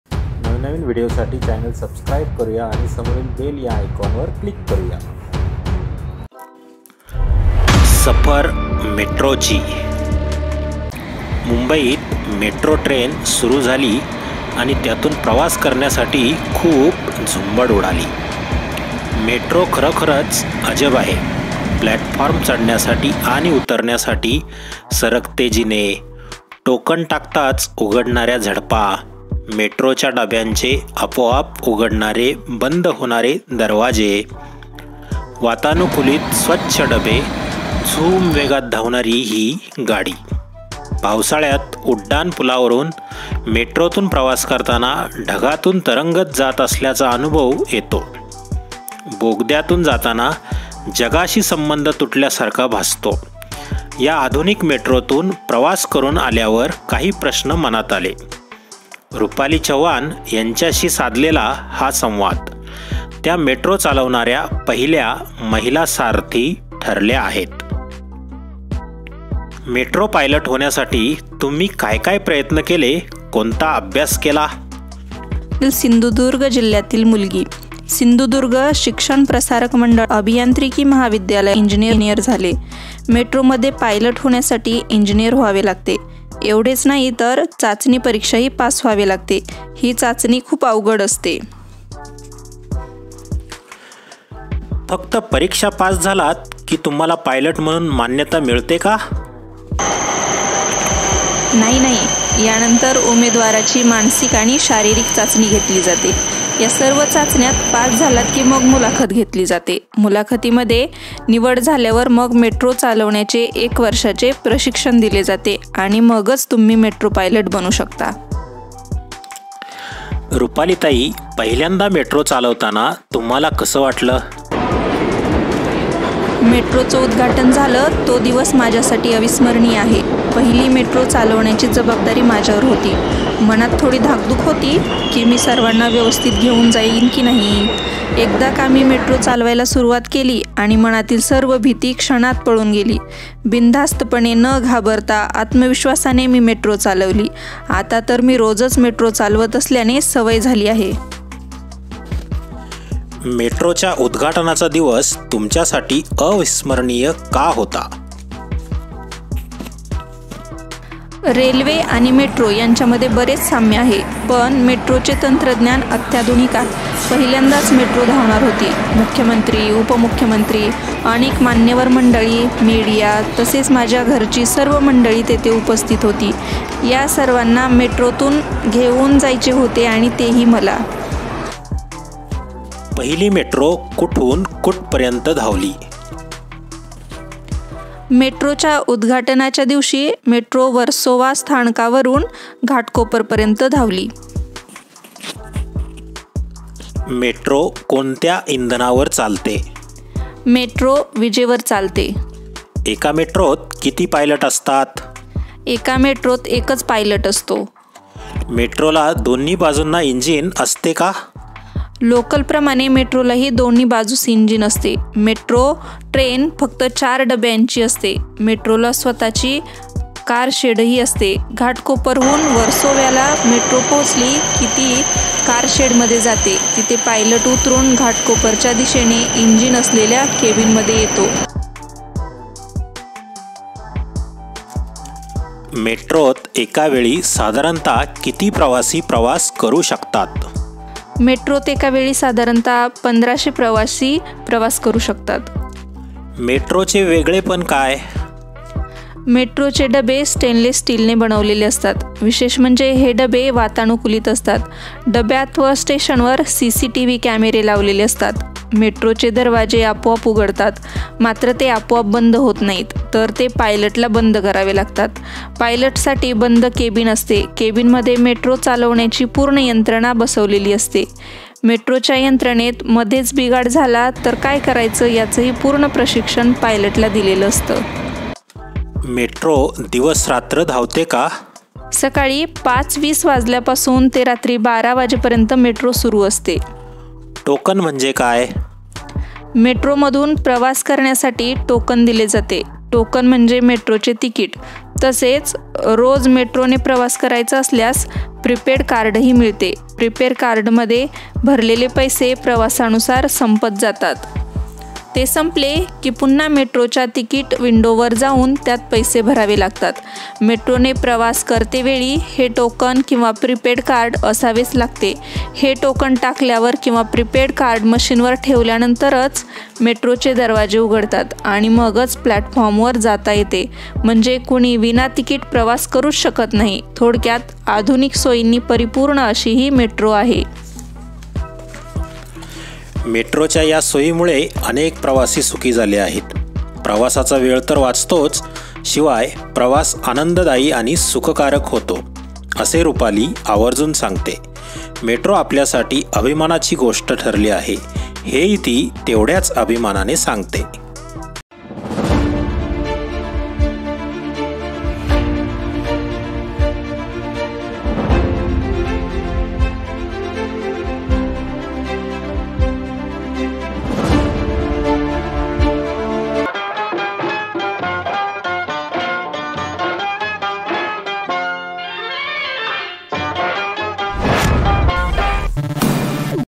सफर मेट्रोची/इयत्ता सहावी मराठी/पाठ १६/Safar Metrochi/Class 6 marathi/Rupali Chavan/१६ सफर मेट्रोची
मुंबईत मेट्रो ट्रेन सुरू झाली आणि त्यातून प्रवास करण्यासाठी खूप झुंबड उडाली. मेट्रो खरोखरच अजब आहे. प्लॅटफॉर्म चढण्यासाठी आणि उतरण्यासाठी सरकते जिने, टोकन टाकताच उघडणाऱ्या झडपा, मेट्रोच्या डब्यांचे आपोआप उघडणारे-बंद होणारे दरवाजे, वातानुकूलित स्वच्छ डबे, झूम वेगात धावणारी ही गाडी. पावसाळ्यात उड्डाण पुलावरून मेट्रोतून प्रवास करताना ढगांतून तरंगत जात असल्याचा अनुभव येतो. बोगदयातून जाताना जगाशी संबंध तुटल्यासारखा भासतो. या आधुनिक मेट्रोतून प्रवास करून आल्यावर काही प्रश्न मनात आले. रुपाली चव्हाण यांच्याशी साधलेला हा संवाद. त्या मेट्रो चालवणाऱ्या पहिल्या महिला सारथी ठरल्या आहेत.
प्रश्न : मेट्रो पायलट होण्यासाठी तुम्ही काय काय प्रयत्न केले. कोणता अभ्यास केला?
रुपाली : मी कोकणातील सिंधुदुर्ग जिल्ह्यातील मुलगी. सिंधुदुर्ग शिक्षण प्रसारक मंडळाच्या अभियांत्रिकी महाविद्यालयातून इंजिनियर झाले. मेट्रोमध्ये पायलट होण्यासाठी इंजिनियर व्हावे लागते. एवढेच नाही तर चाचणी परीक्षाही पास व्हावे लागते. ही चाचणी खूप अवघड असते.

१६ सफर मेट्रोची
सफर मेट्रोची/इयत्ता सहावी मराठी/पाठ १६/Safar Metrochi/Class 6 marathi/Rupali Chavan